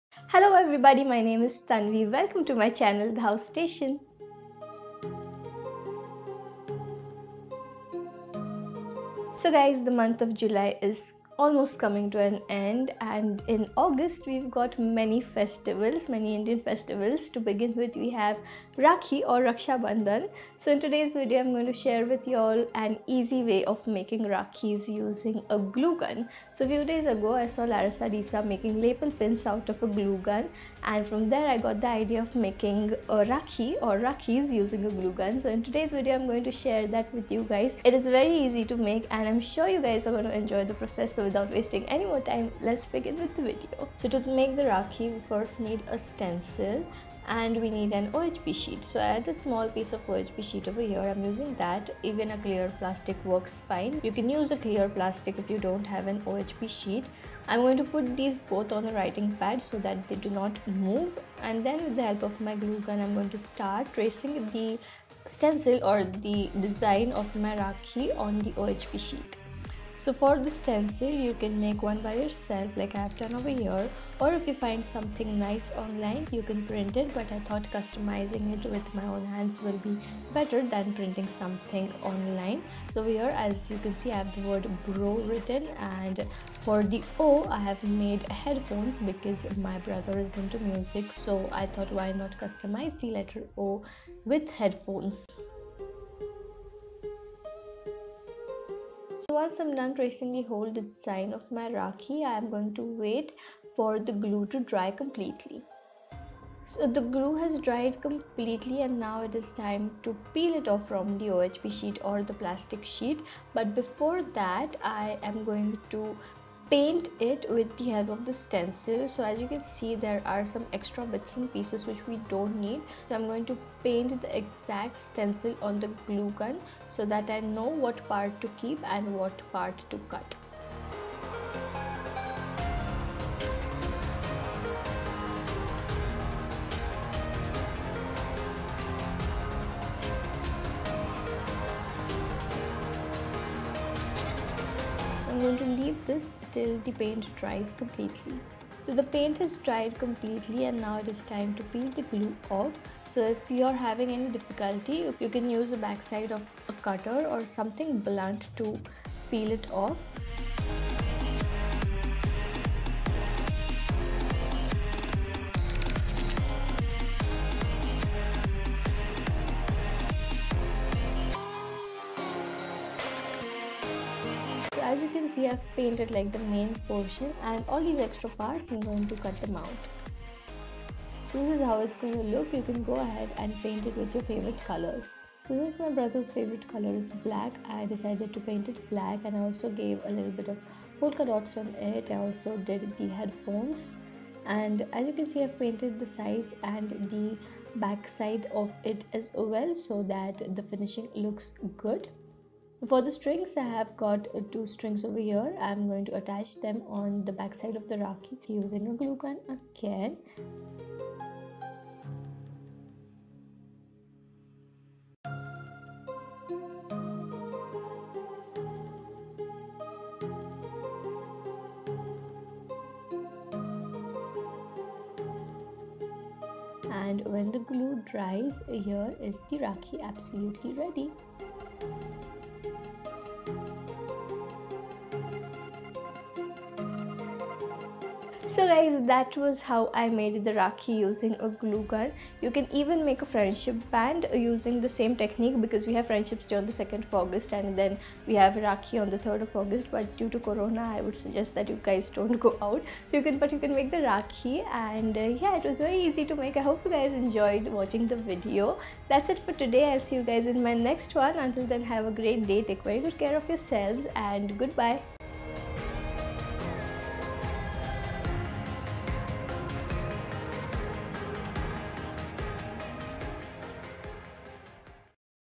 DIY: Glue Gun Rakhi
Getting creative for this video with making a rakhi out of hot glue using a glue gun. It can be customized as per your liking and is so much fun to make. I am sure you are going to enjoy it.

xoxo,
Tanvi